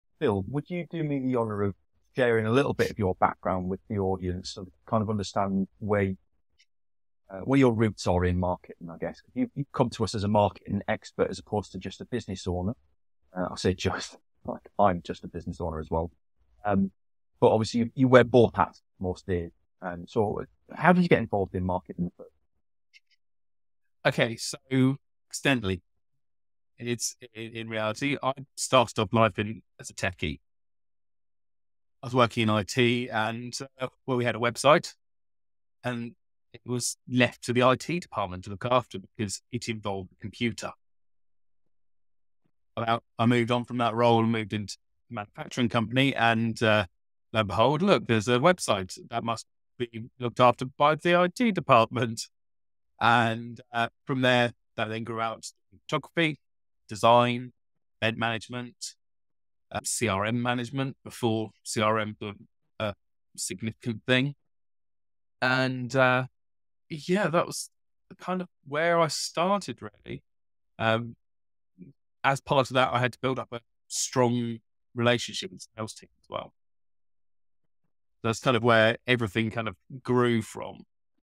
Marketing Genius Phil's Journey from IT to Marketing Guru | Marketpulse Episode 8
Full Episode Drops 1st of May 2024!!

Explore the secrets of marketing success with our latest MarketPulse episode featuring Phil Bacon, a master marketer with an extensive background spanning from IT to groundbreaking marketing strategies. This episode, hosted by Paul Banks, delves into the intersections of marketing and sales, offering invaluable insights from Phil’s rich career and innovative approaches.

Phil discusses the evolution of the marketing funnel and how the merging of sales and marketing has transformed business strategies. He addresses the changing customer journey in the digital age, where self-research has become increasingly significant, affecting how marketers need to approach their campaigns. Listen to Phil's compelling stories, from handling major brands to navigating the challenges of marketing in various industries.

Key Takeaways:

Insights into the integration of sales and marketing
Strategies for navigating the modern customer’s research-driven journey
Phil’s unique approach to blending digital and traditional marketing tactics
Key Topics Covered:

The synergy of sales and marketing
Evolution of customer interactions and expectations
Leveraging traditional marketing tactics in the digital era
Get a glimpse of the expert strategies that have shaped Phil’s career and continue to influence the marketing landscape today.